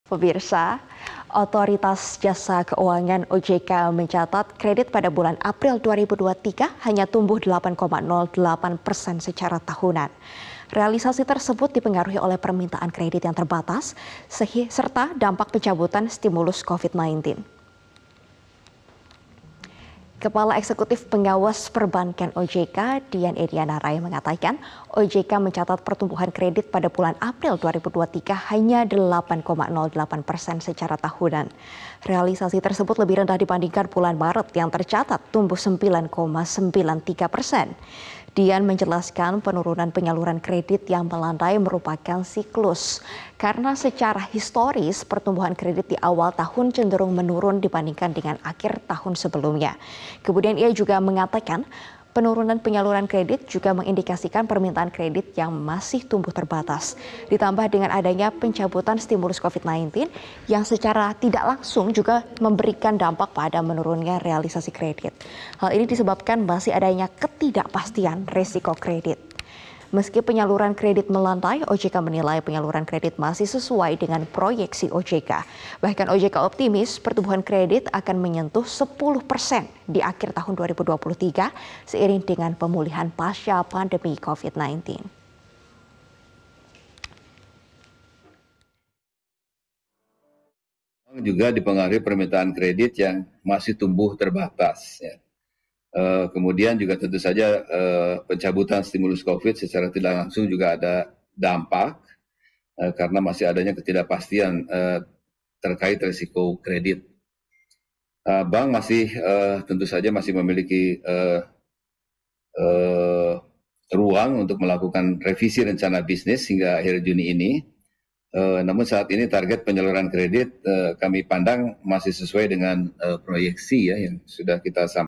OJK: Kredit Perbankan Tumbuh 8,08% di April 2023
MetroTV, OJK mencatat kredit bulan April 2023 hanya tumbuh 8,08% secara tahunan. Realisasi tersebut dipengaruhi oleh permintaan kredit yang terbatas serta dampak pencabutan stimulus Covid-19.

Kepala Eksekutif Pengawas Perbankan Dian Ediana Rae mengatakan, OJK mencatat pertumbuhan kredit pada April 2023 hanya 8,08% secara bulanan. Realisasi tersebut lebih rendah dibandingkan Maret 2023 yang tercatat tumbuh 9,93%.